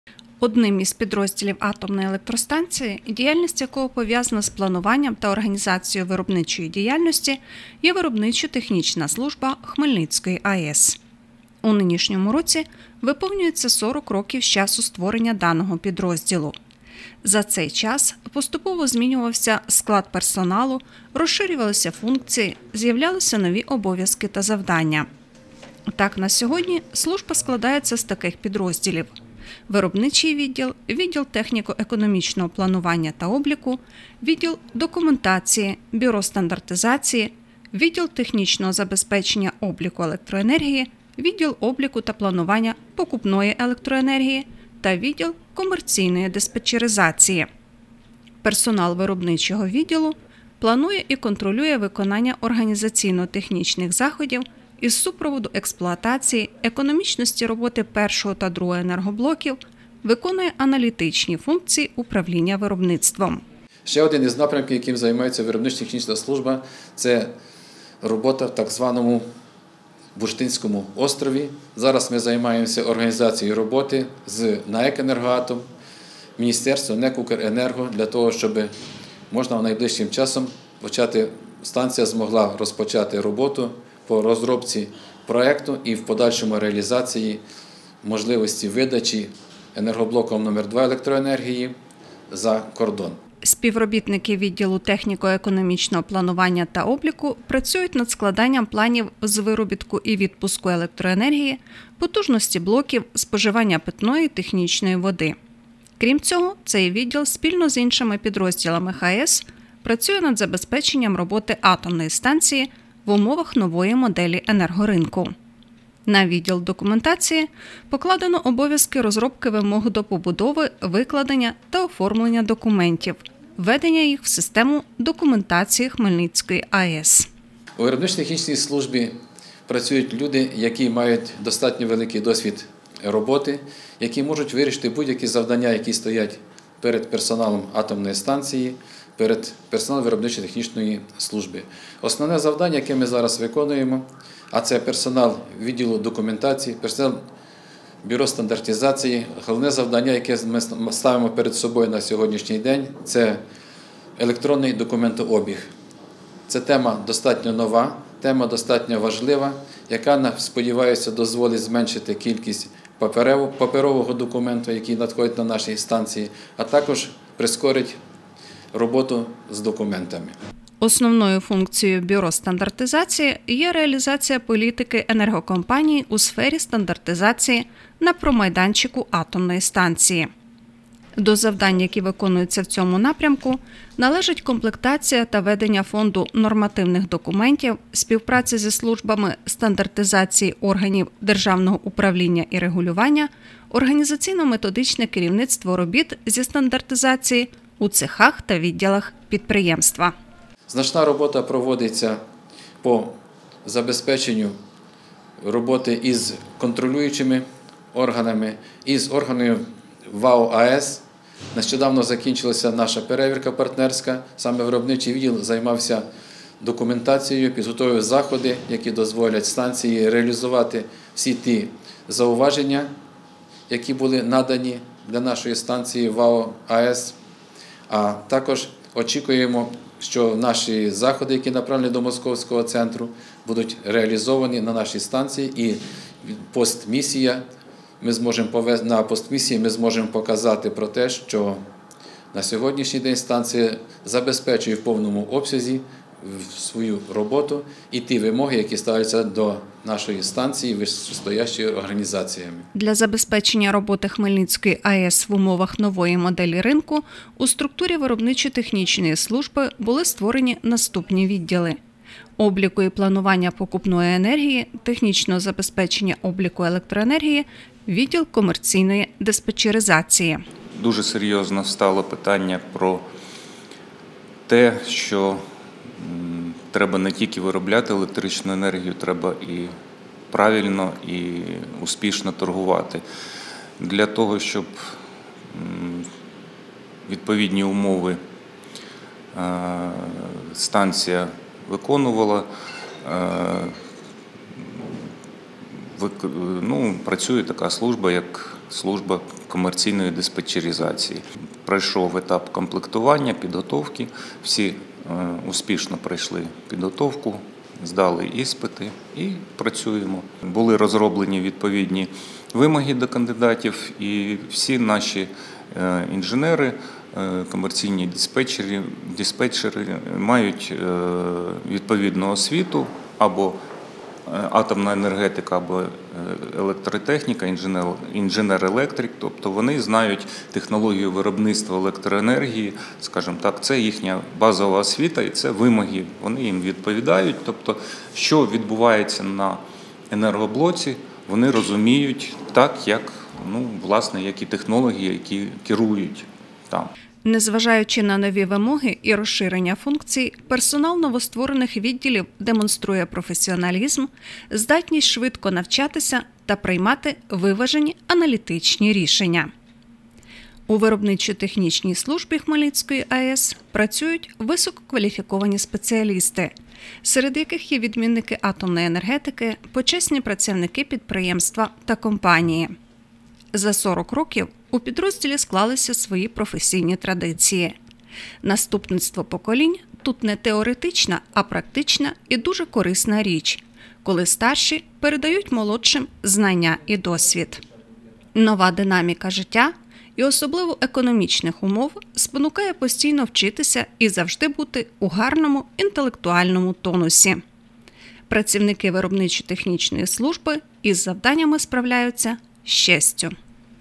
28.02.2020.ХАЕС. Виробничо-технічній службі – 40 років.
Василь Летвинко – про актуальні завдання підрозділу.